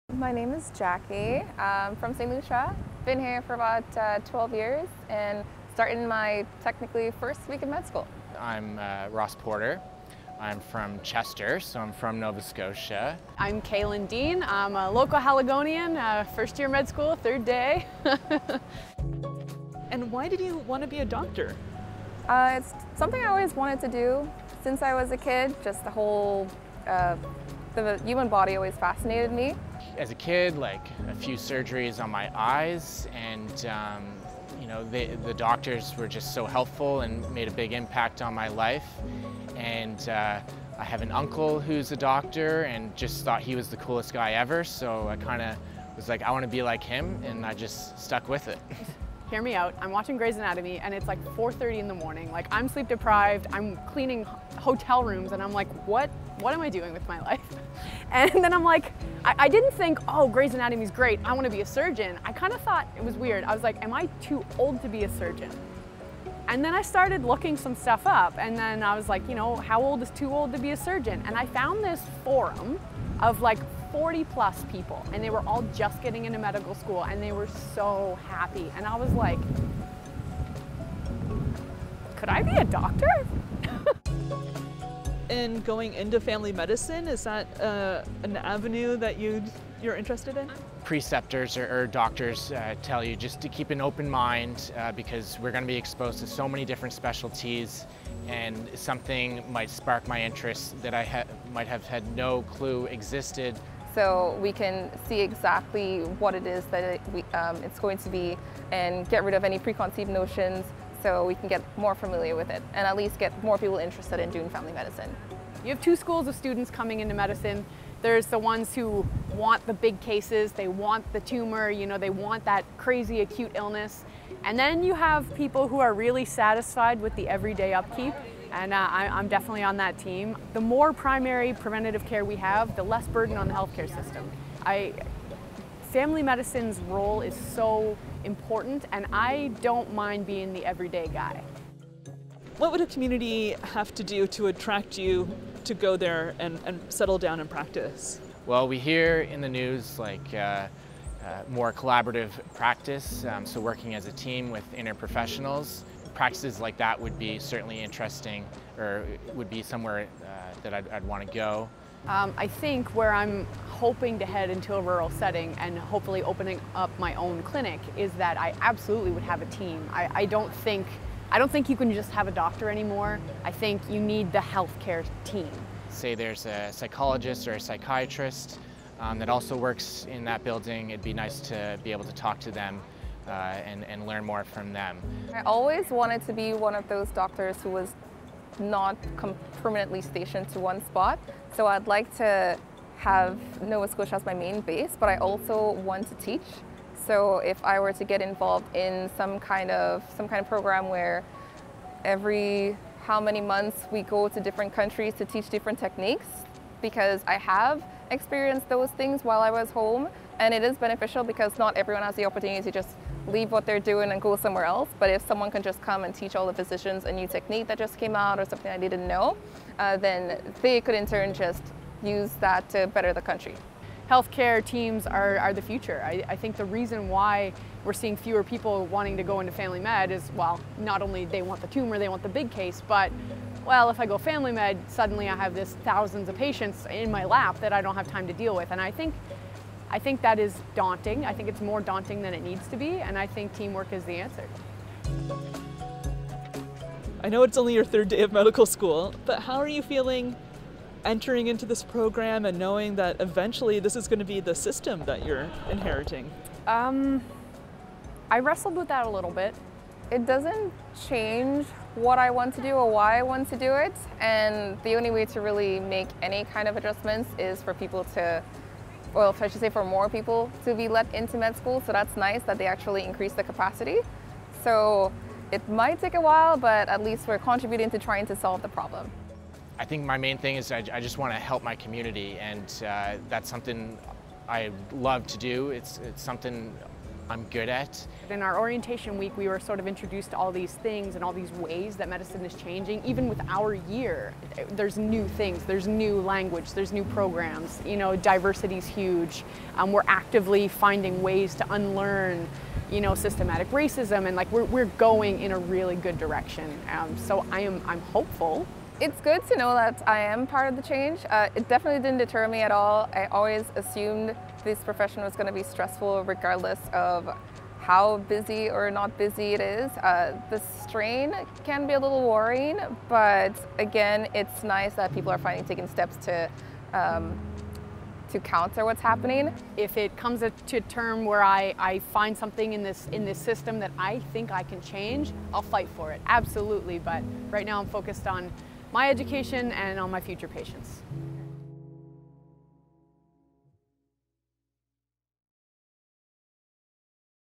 3 Halifax med students open up about their hopes and concerns for the future
The largest medical school class in Dalhousie University history started class this week in the midst of Nova Scotia's ongoing health-care crisis. Three students give their views.